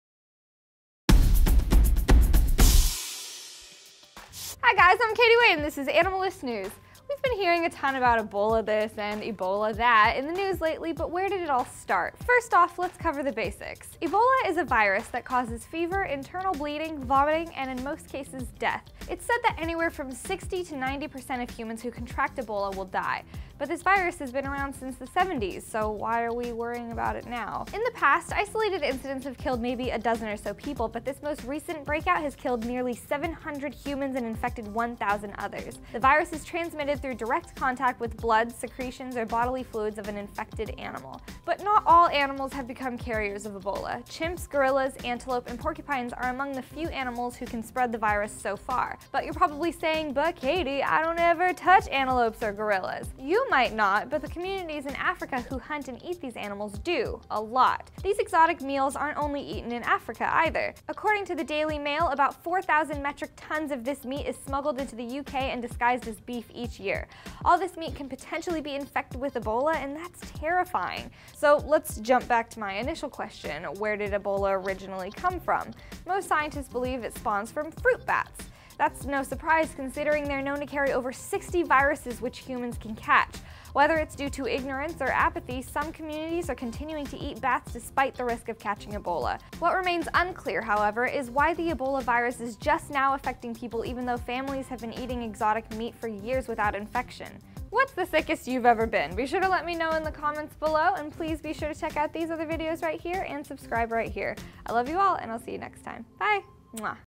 Which Animals Spread Ebola?
The Ebola virus is currently running rampant in West Africa, taking nearly 1000 lives in what is being considered it’s worst outbreak yet. It’s known that some animals can be carriers of the disease, but which one? How are these animals potentially spreading the disease?